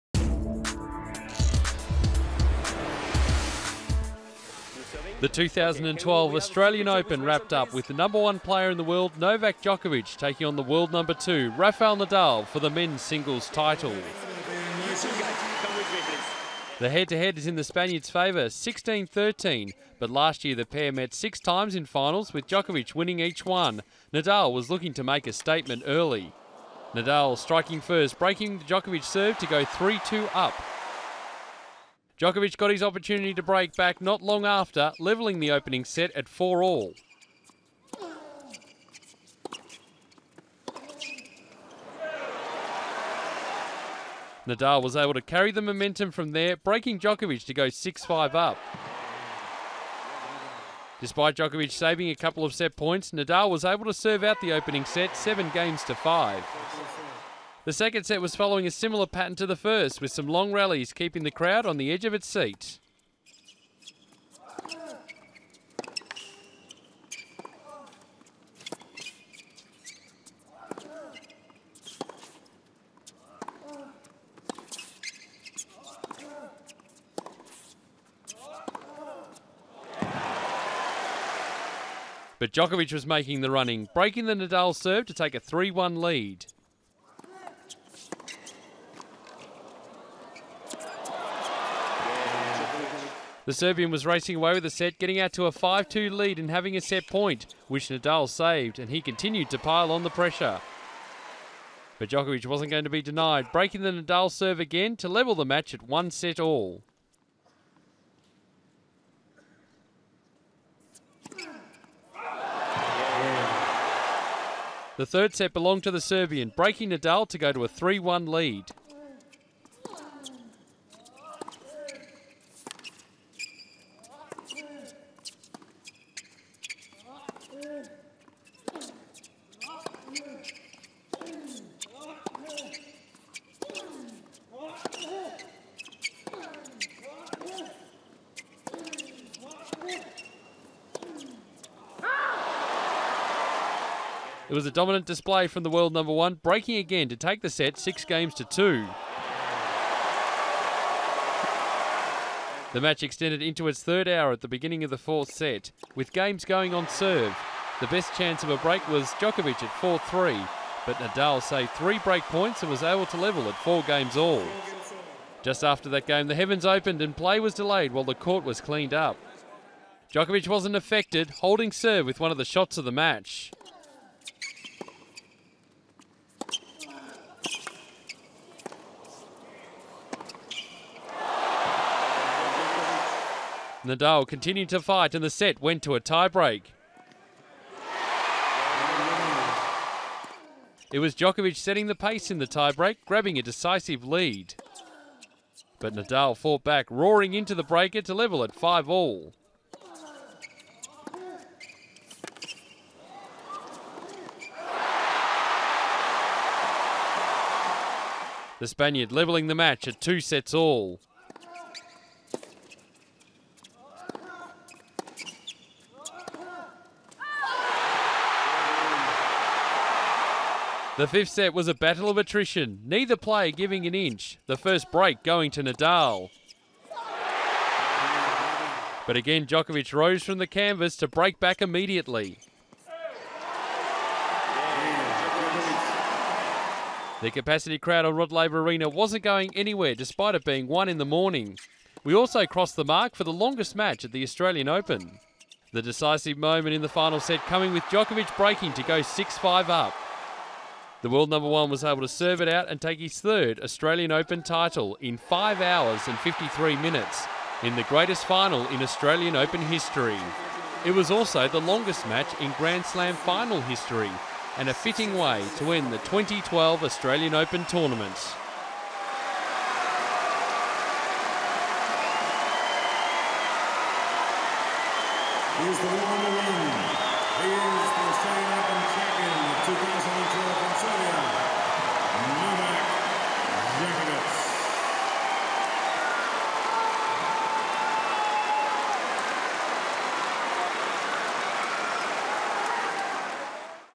Novak Djokovic vs Rafael Nadal - The Greatest Final Ever! | Australian Open 2012
Novak Djokovic wins the longest Grand Slam final in history, overcoming Rafael Nadal in an epic five-set encounter at the Australian Open 2012.

Australian Open 2016 from 19 January - 1 February 2016.